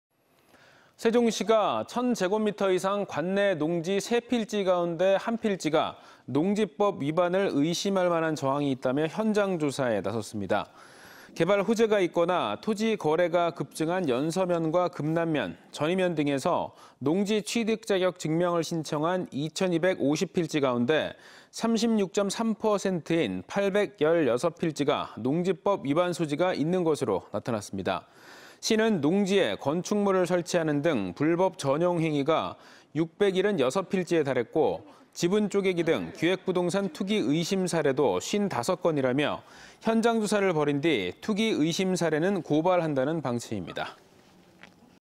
[21.04.12] 세종 농지 3필지 중 1필지 농지법 위반 의심｜ TJB 대전·충남·세종뉴스
세종시가 천 제곱미터 이상
농지 3필지 가운데
1필지가 농지법 위반을
의심할 만한 정황이 있다며
현장 조사에 나섰습니다.

개발 호재가 있거나 토지 거래가 급증한
연서면과 금남면, 전의면 등에서
농지취득자격 증명을 신청한
2천 250필지 가운데
36.3%인 816필지가
농지법 위반 소지가 있는 것으로 나타났습니다.

시는 농지에 건축물을 설치하는 등
불법 전용 행위가 676필지에 달했고,
지분 쪼개기 등
기획부동산 투기 의심 사례도 55건이라며
현장 조사를 벌인 뒤
투기 의심 사례는 고발할 방침입니다.